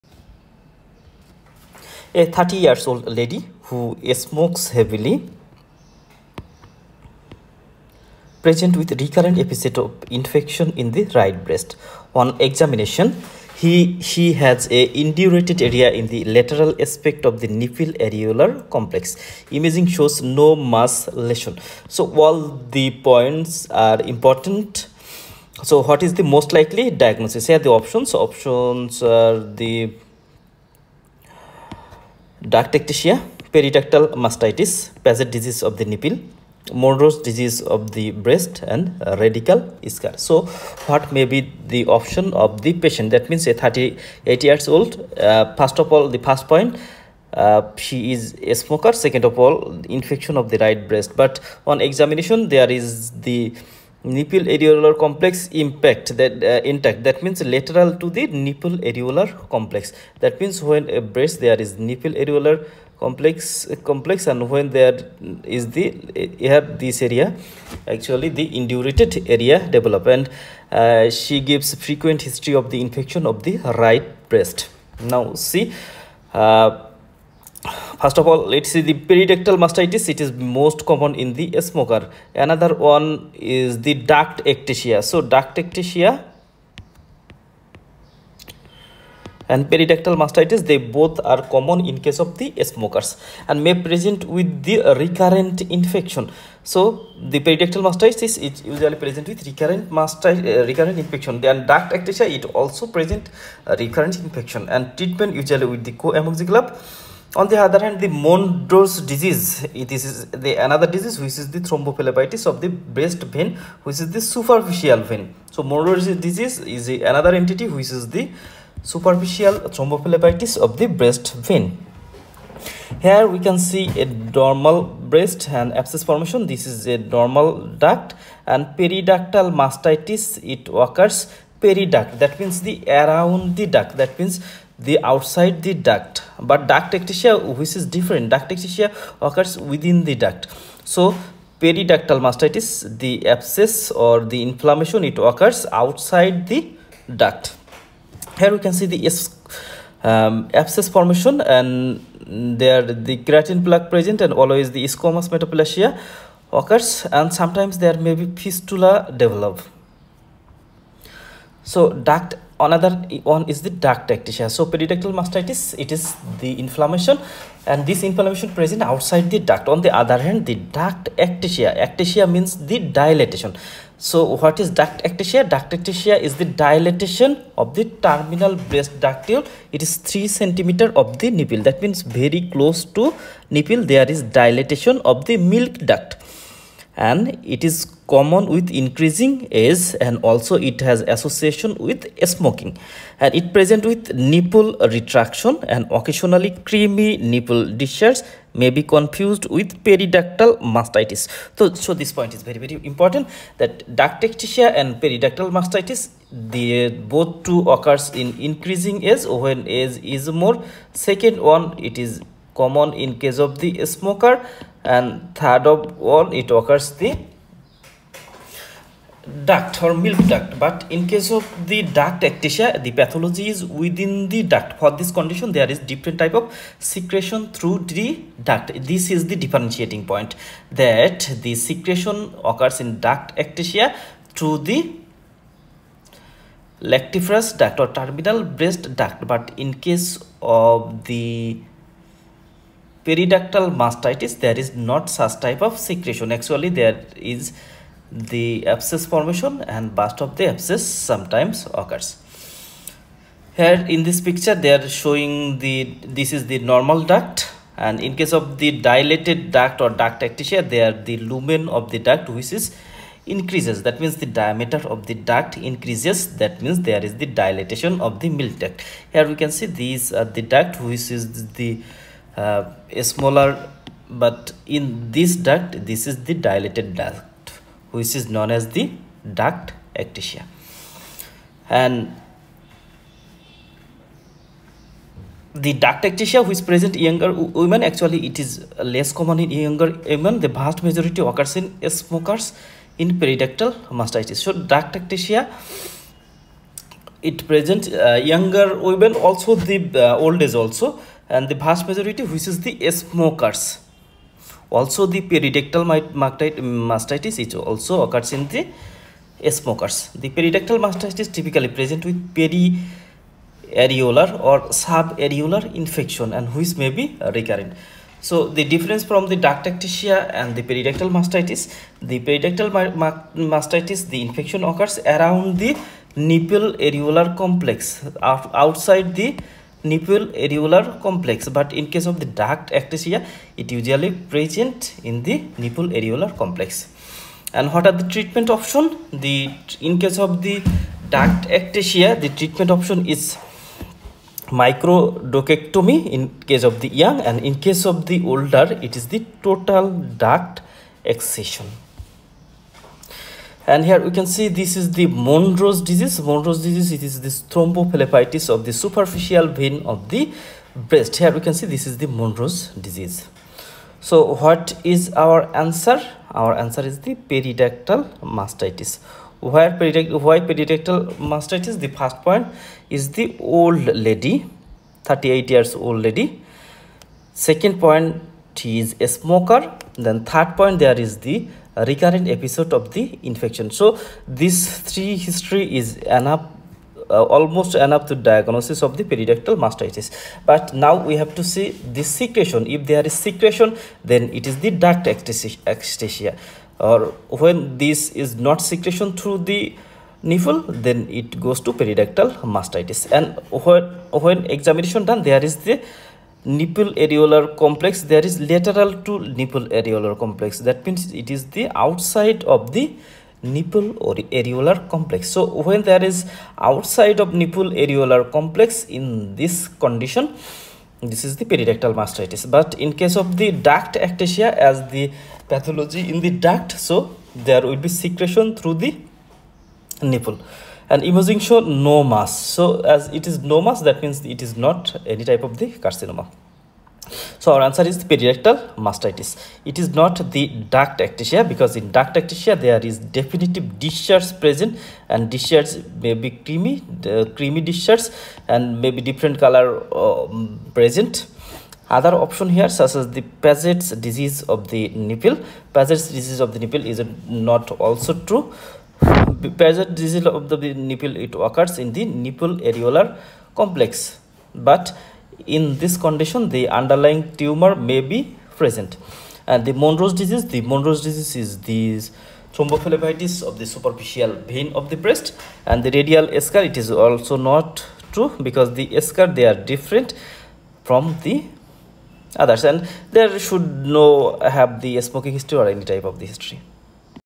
Breast pathology MRCS question solve (CRACK MRCS)
it includes
1. eMRCS videos
2. Fawzia sheet solved
3. Recalls and mores